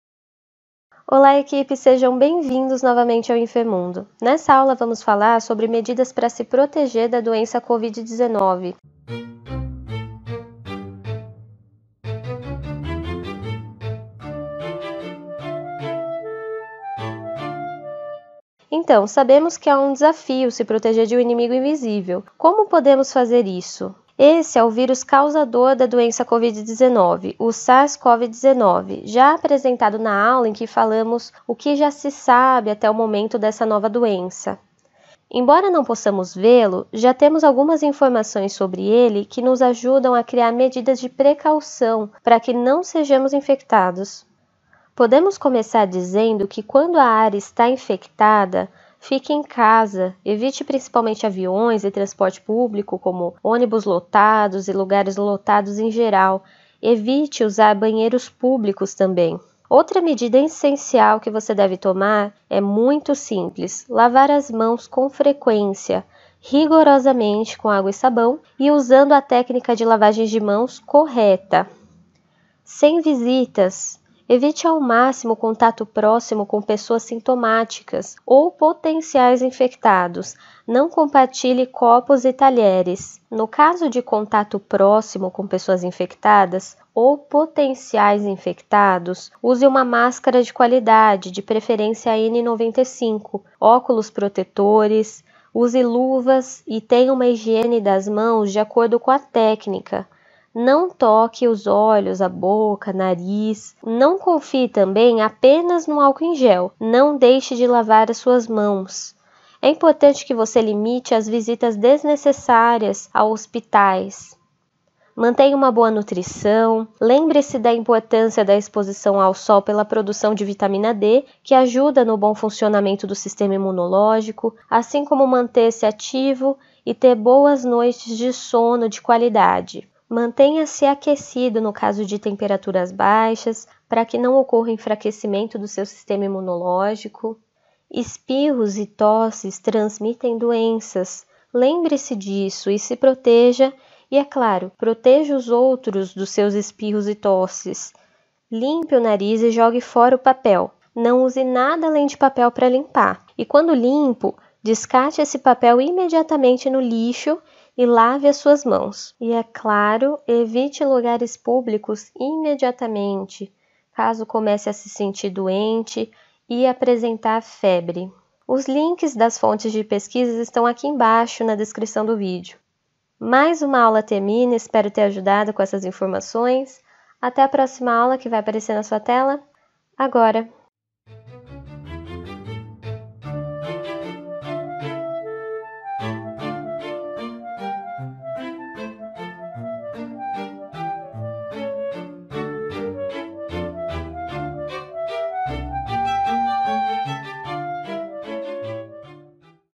COVID-19 | COMO SE PROTEGER EM ÁREAS INFECTADAS?
Olá equipe! Sejam bem-vindos novamente ao Enfermundo!

Nessa aula vamos falar sobre medidas de precaução para não contrair o vírus causador da doença COVID-19.

Espero tê-los ajudado com mais esse vídeo, até a próxima!